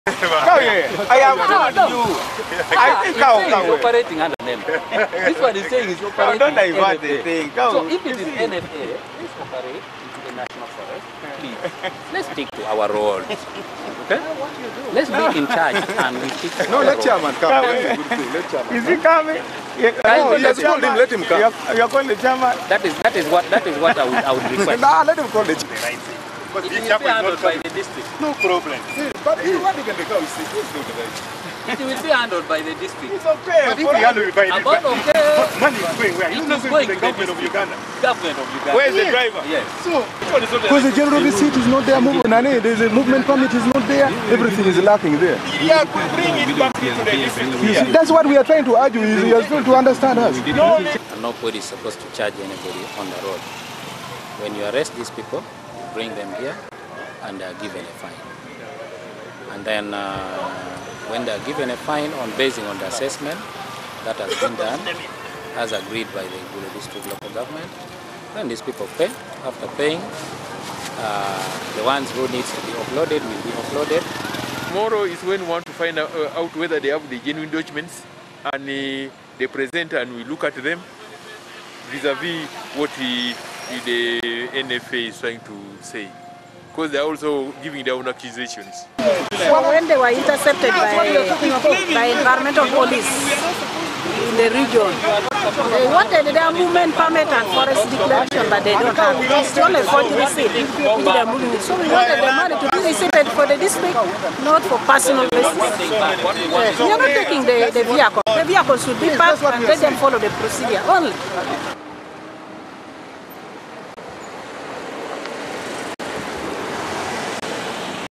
GULU CHARCOAL ROW: Police, forestry and district officials clash
The District Administrative Officer of Gulu Milton Kato has ordered for the impounding of trucks intercepted by the National Forestry Authority while ferrying charcoal. The order to impound the trucks follows a clash between officials from the National Forestry  Authority and Gulu local government over the payment of taxes for trading in charcoal.